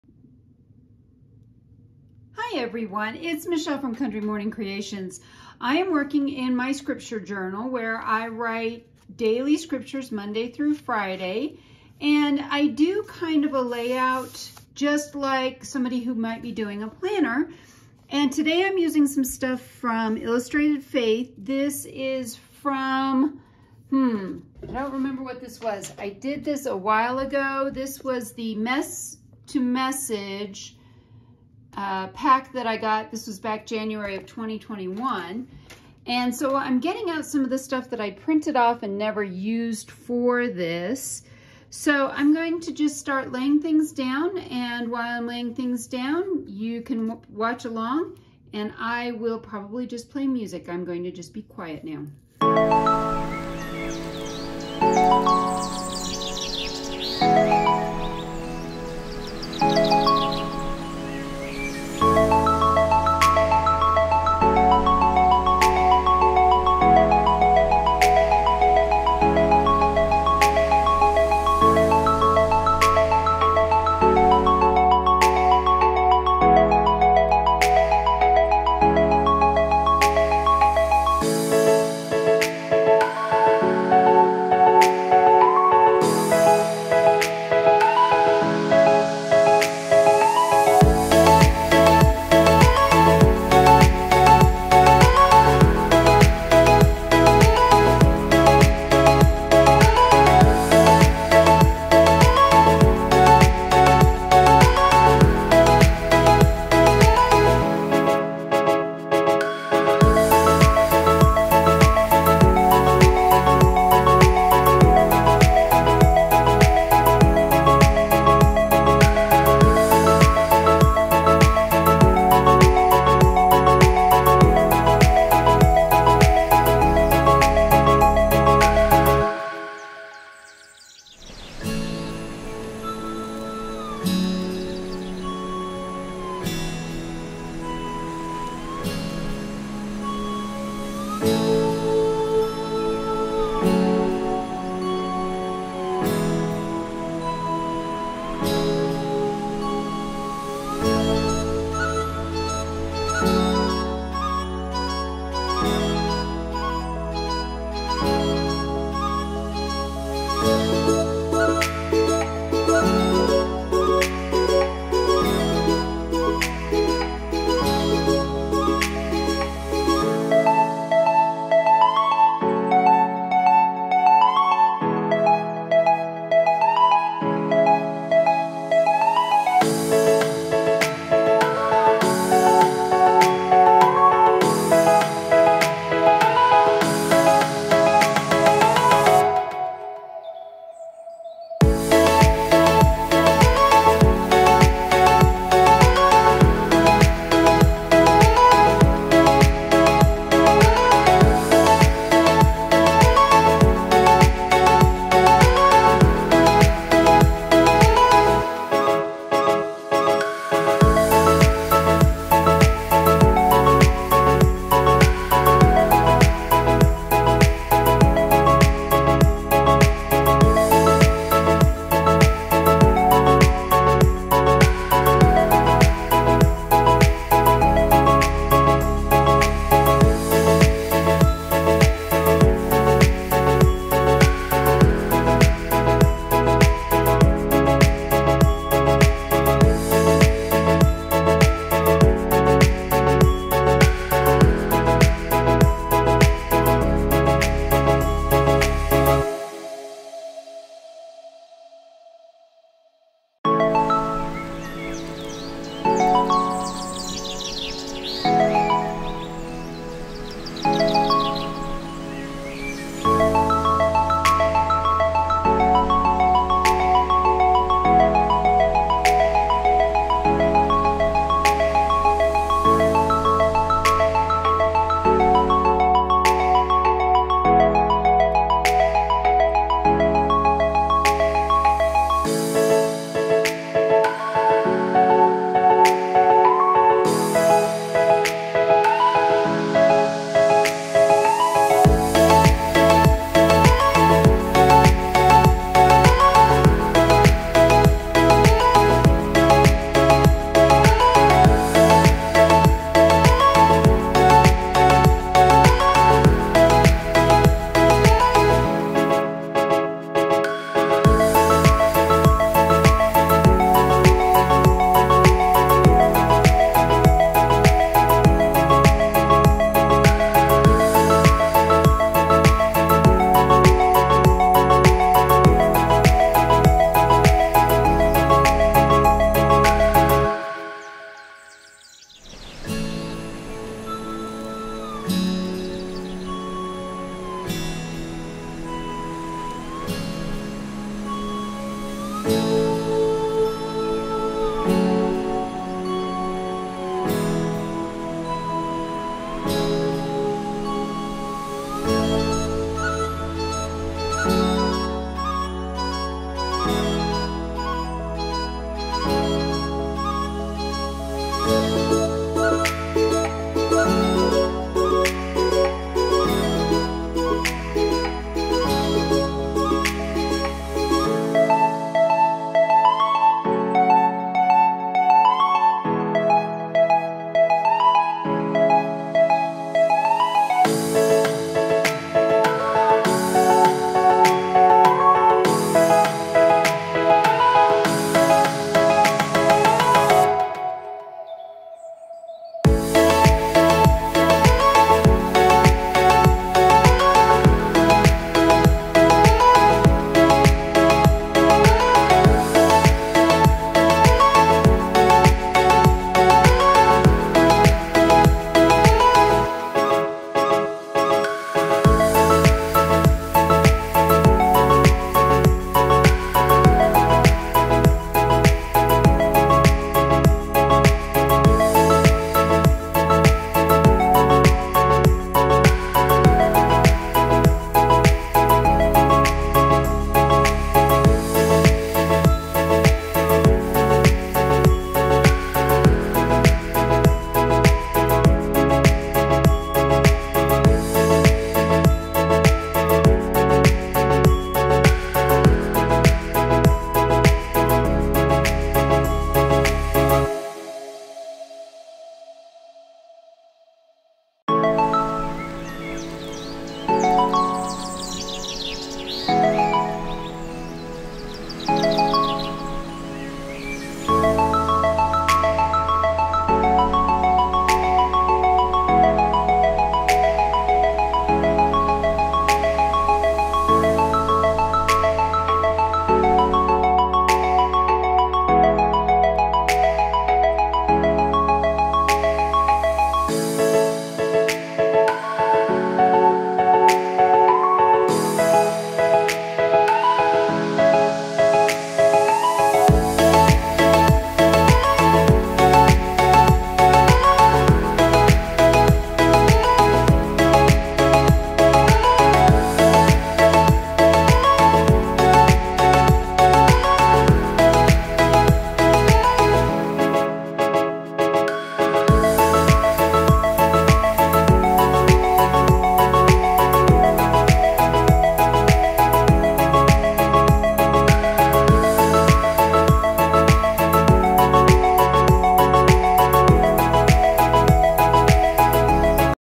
Week four of my May scripture layouts.
This week I am using up pieces from my Illustrated Faith January Bible study called Mess to Message. I love the colors in this kit. Follow along as I create my planner style layout for writing my scriptures.

Monthly Journal Class